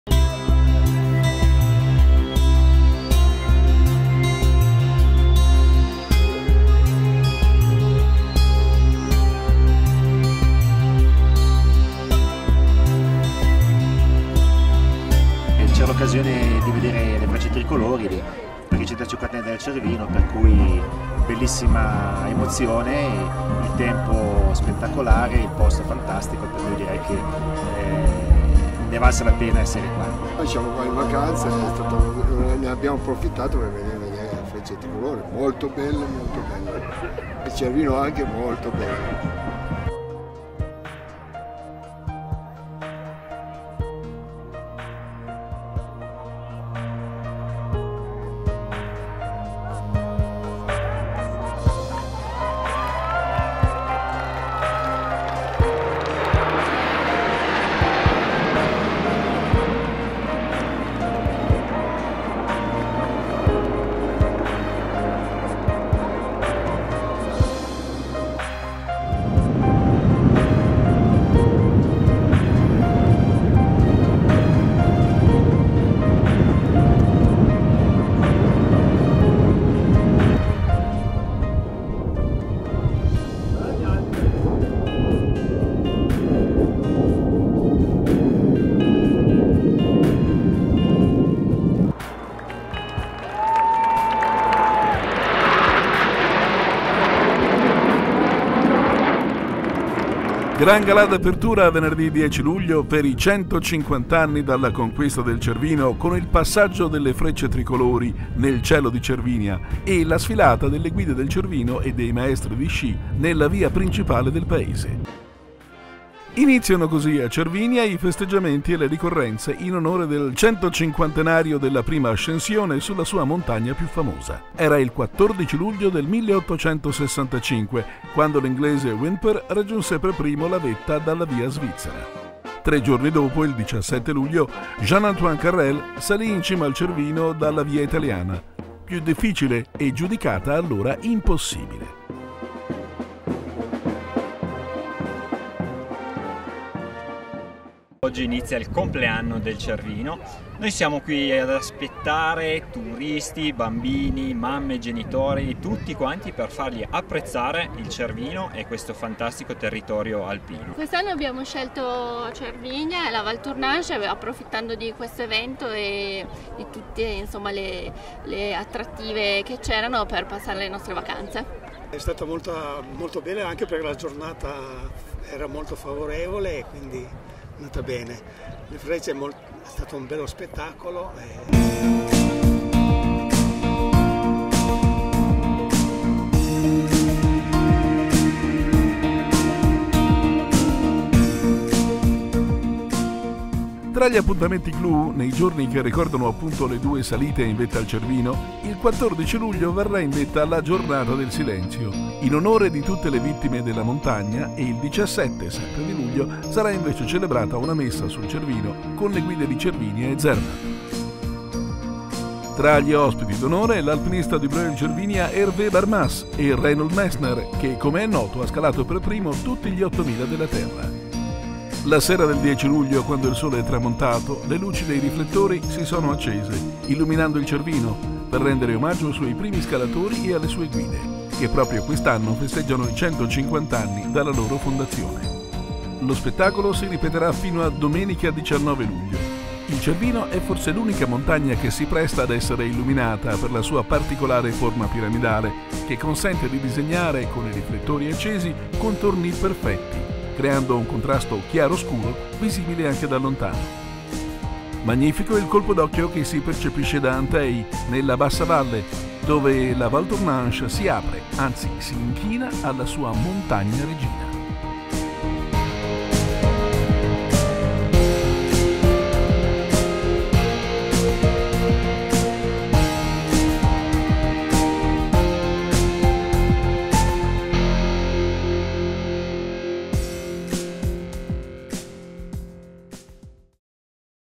Breuil-Cervinia celebra i 150 anni della prima salita sul Cervino.
Al via i festeggiamenti per ricordare i 150 anni dalla prima salita sul Nobile Scoglio.